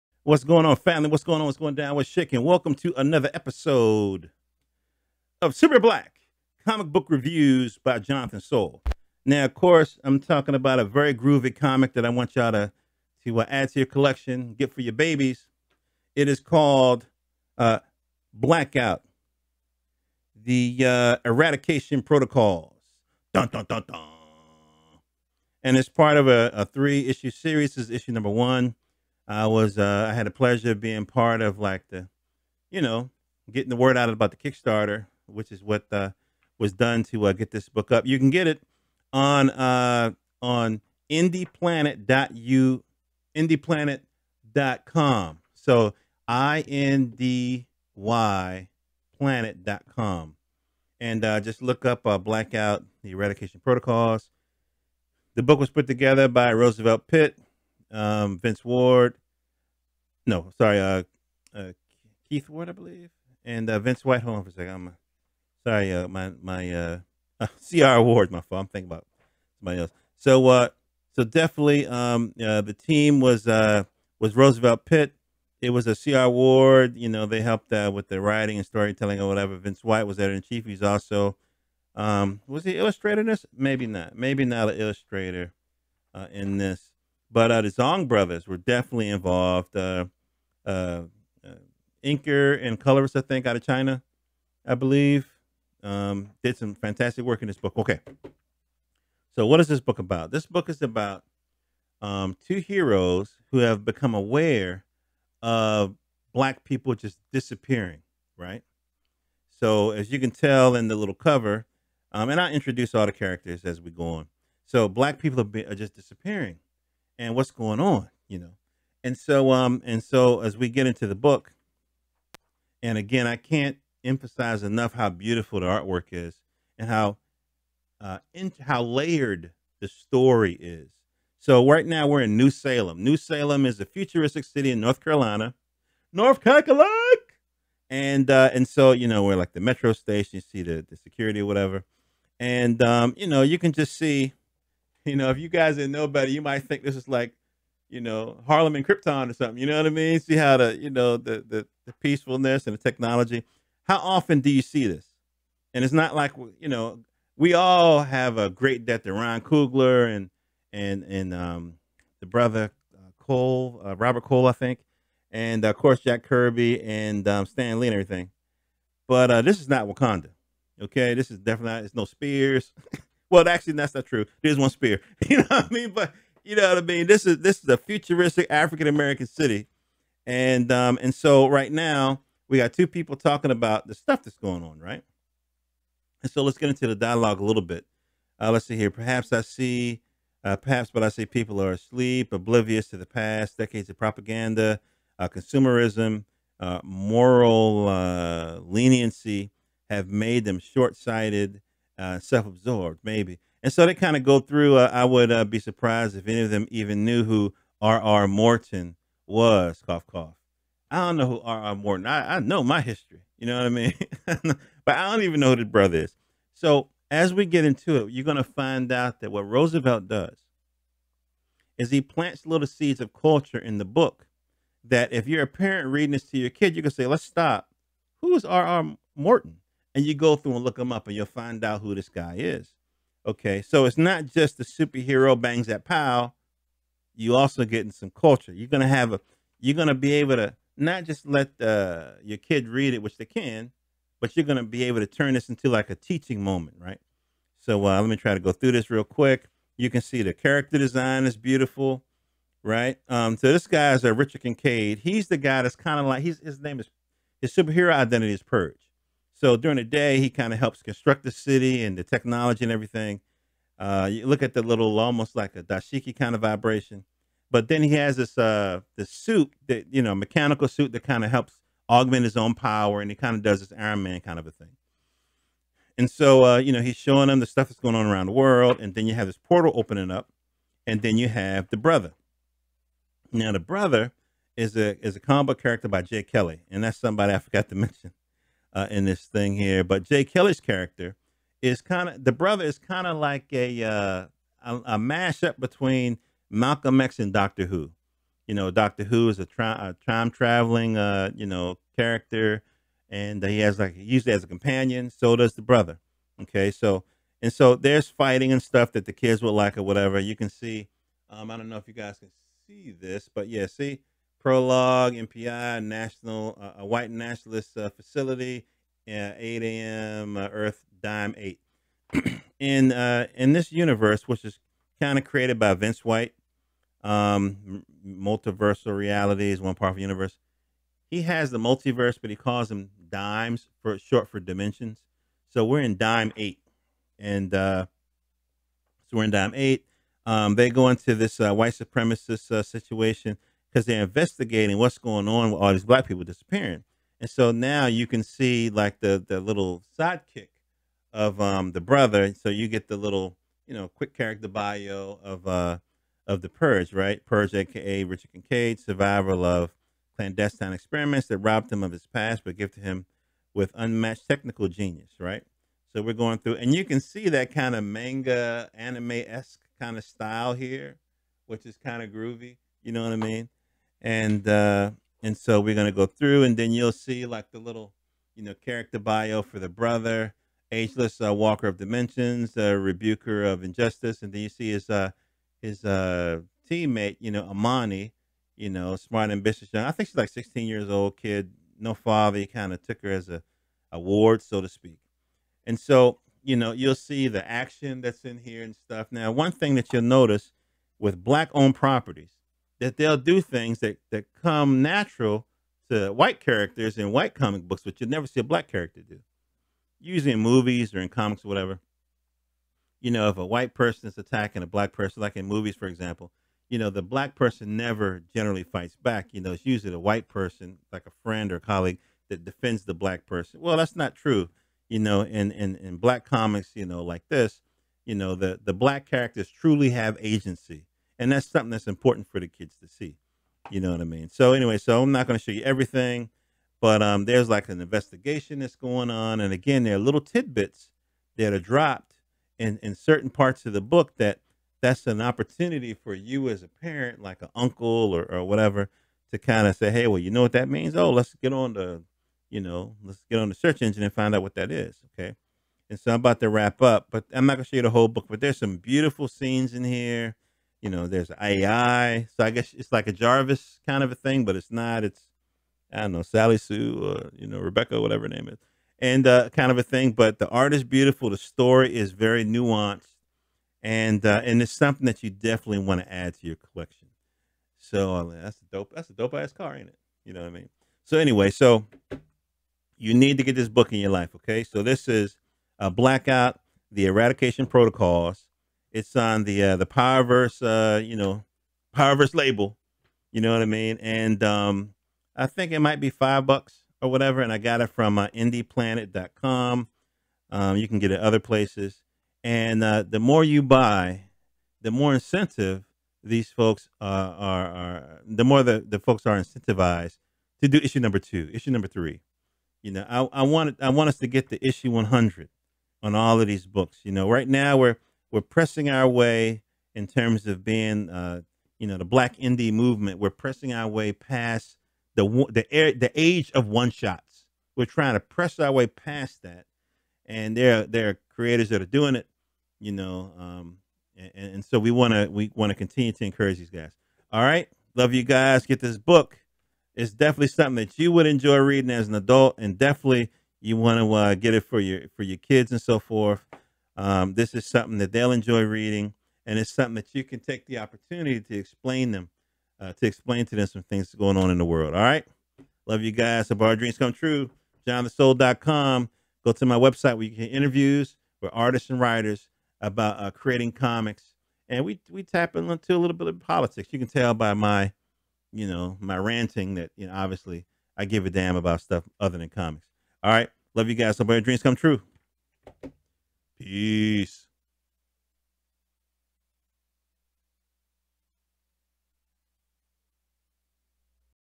COMIC REVIEWED: Black Out the Eradication Prootocols
Our heroes investigate why Black people are disappearing from the timeline. Purge (cybernetic), the Brother (time traveler) team up to fight an extradimensional threat. First issue of three part series. Enjoy the review.